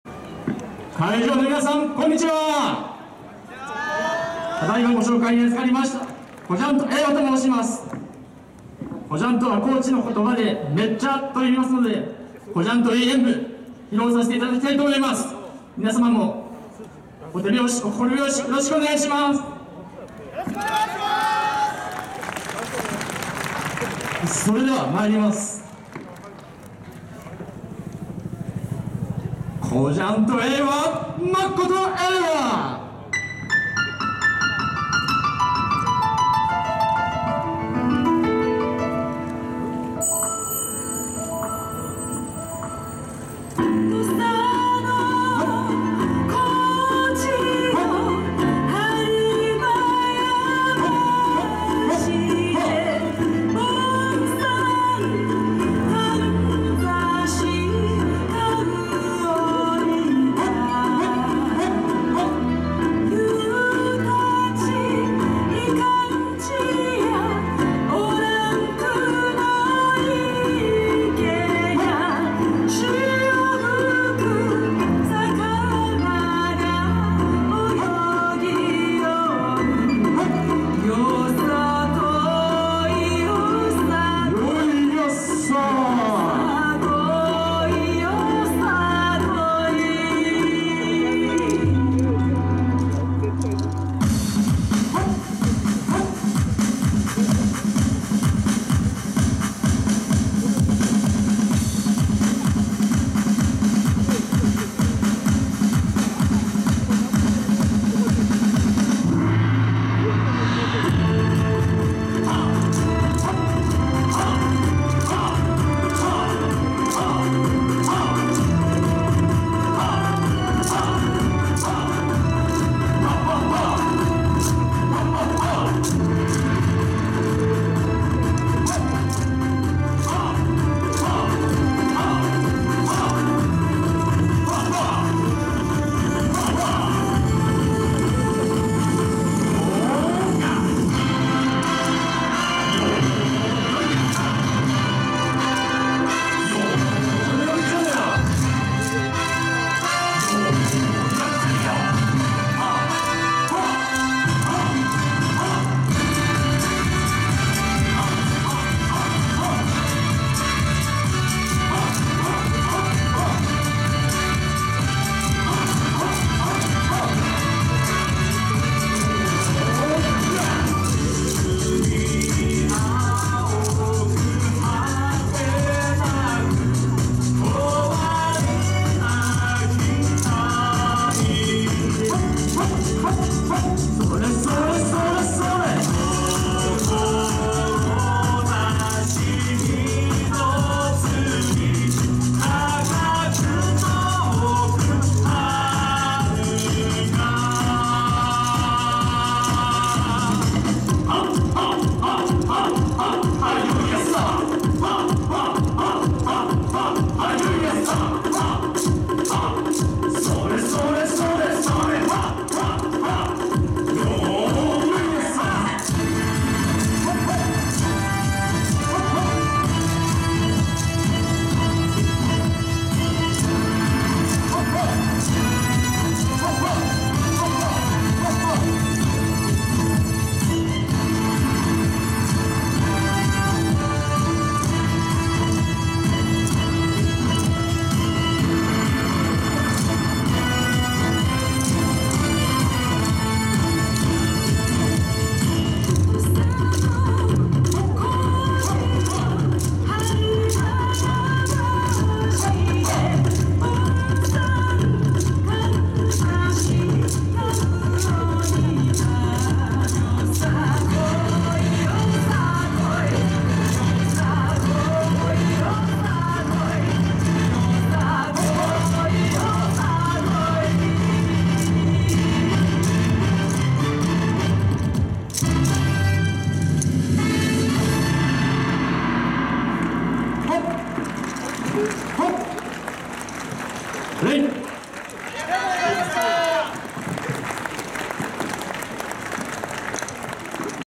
サクラタウンよさこい21 こじゃんとrEIWA 2回目
21-12-12 サクラタウンよさこい2021～師走祭～
所沢市～サクラタウン～「COOL JAPAN FOREST 構想」の拠点施設
千人テラスステージ
＊画面が暗くなっています。申し訳ございません。